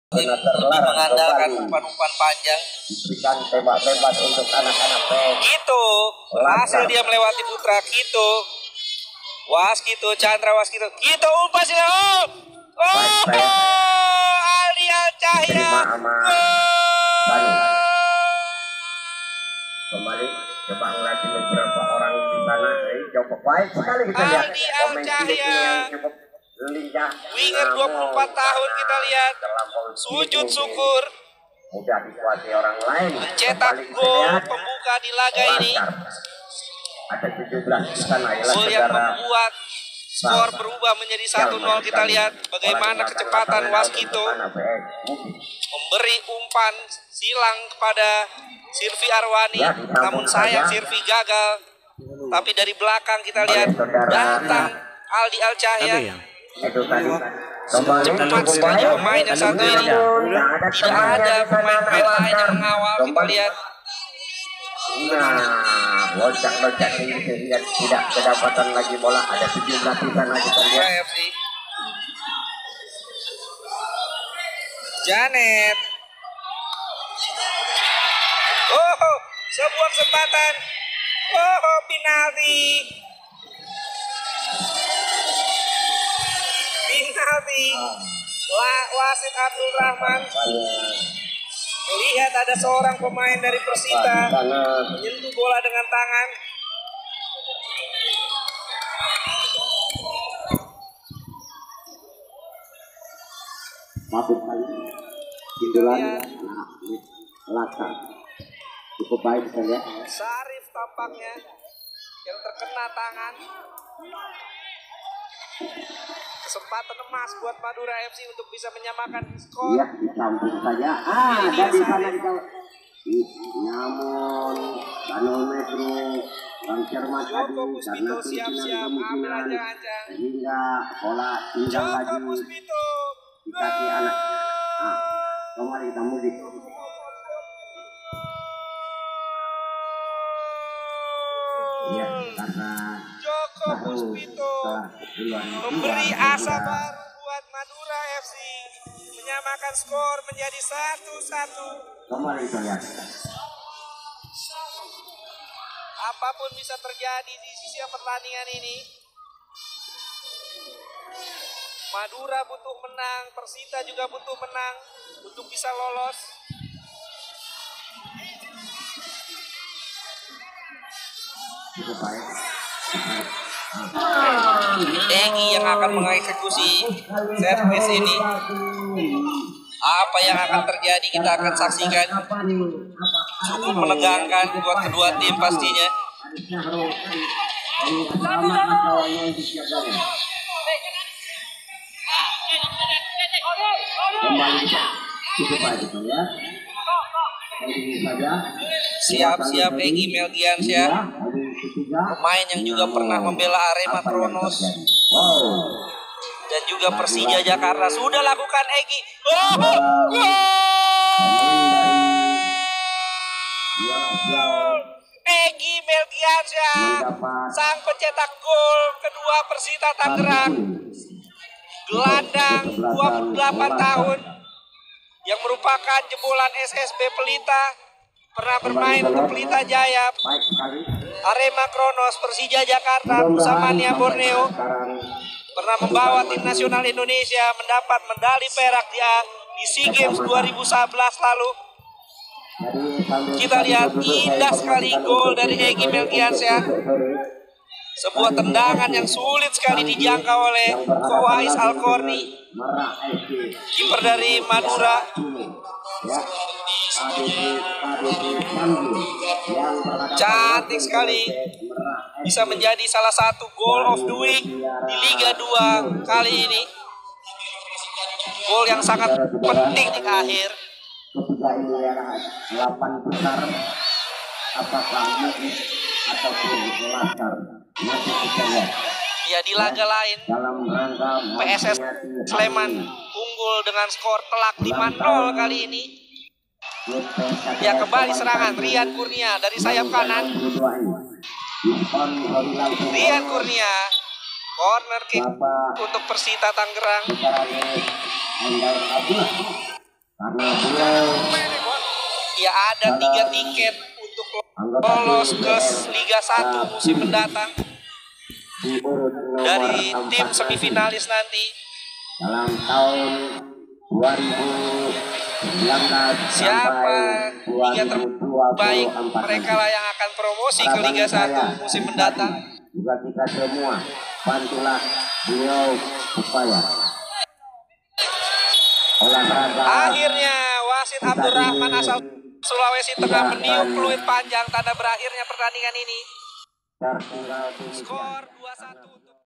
(Fulltime) Madura Fc 1 vs 2 Persita, cuplikan gol dan highlights pertandingan babak 8 besar liga 2
Persita akhiranya berhak menemani pss sleman untuk menuju semifinal di laga pamungkas babak 8 besar liga 2